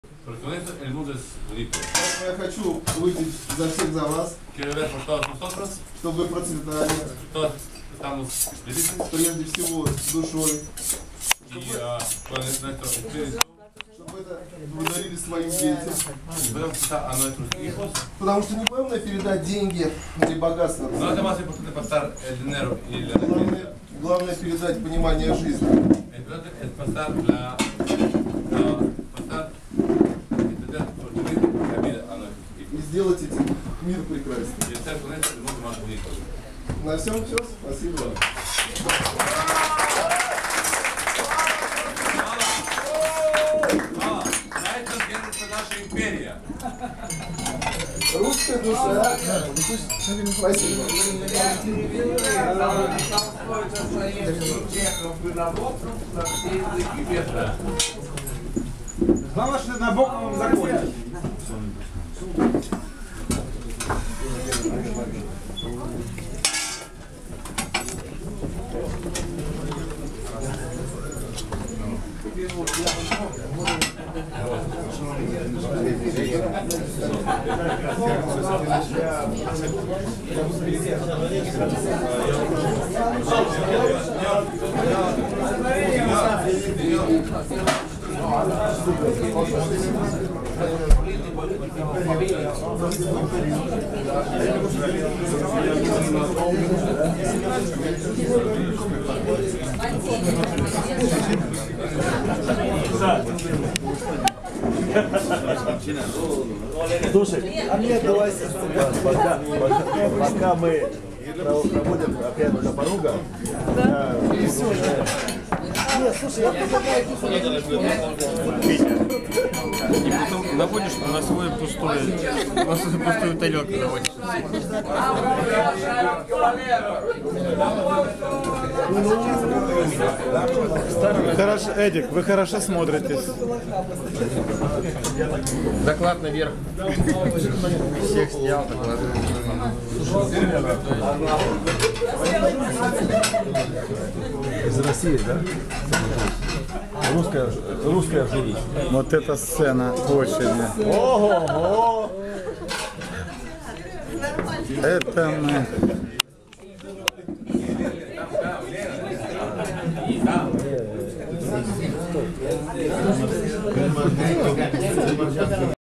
Тост от масонской ложи Магуро, в исполнении Славы.
Гросмастер Вячеслав произносит тост под ритуально убиенного быка, во здравие наших общих дел свободных каменщикофф.